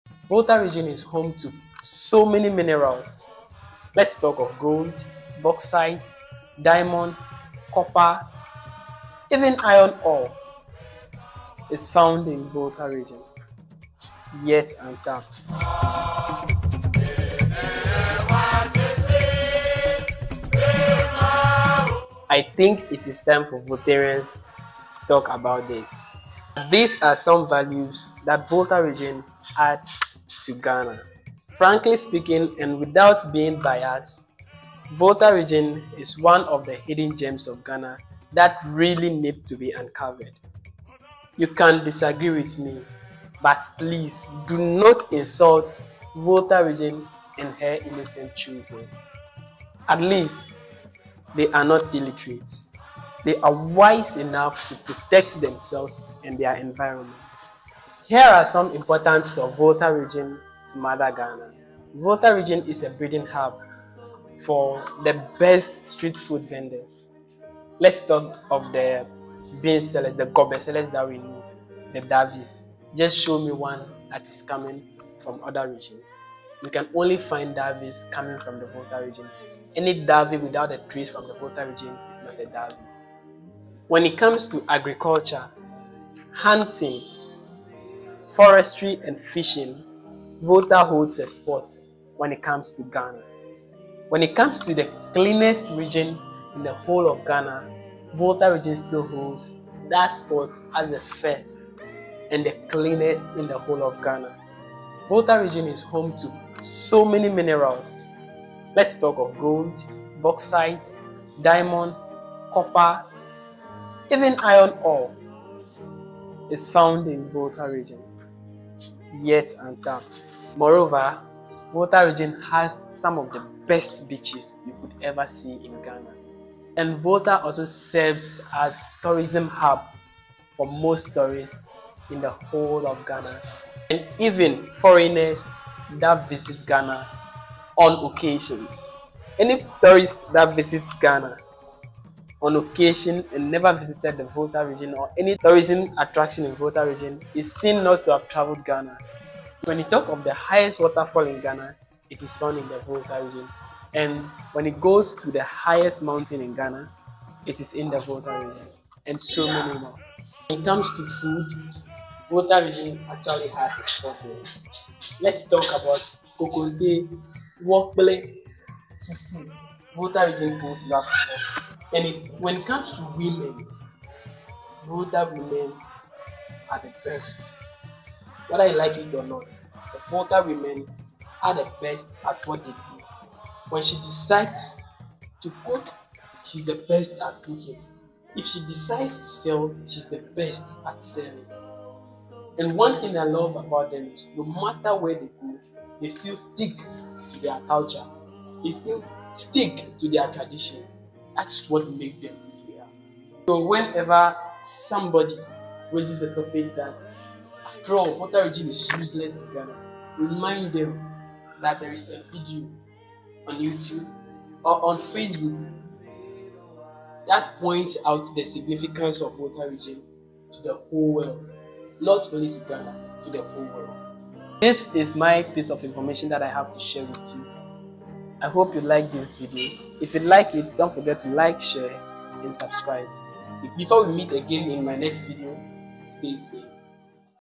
Does Volta Region 🇬🇭 have natural resources? The Importance of Volta Region to The World.
Many see Ghana’s Volta Region as useless because Volta is perceived to lack Natural resources, Because Volta is less Developed many underrate the importance of Volta Region to Ghana 🇬🇭. Here are some values that Volta add to Ghana hence her importance to mother Ghana 🇬🇭  and Africa at large.

Be part of my awesome Family on All Social Media Platforms: @sermonquoffie